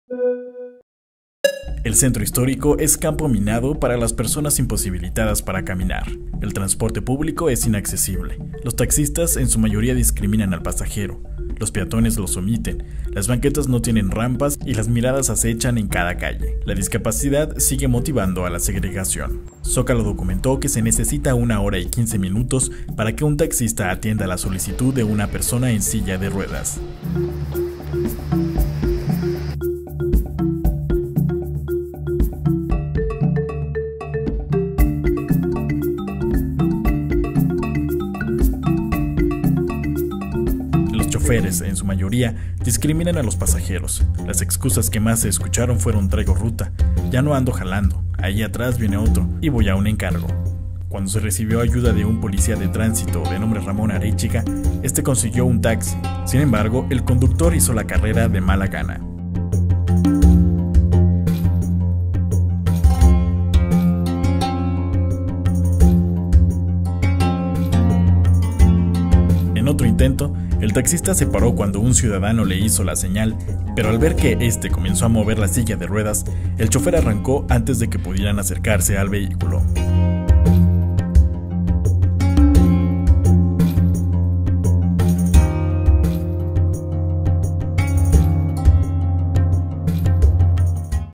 Es un desafío ir en silla de ruedas en el centro de Saltillo
Discriminación y burla soportan las personas con movilidad restringida en el centro de Saltillo.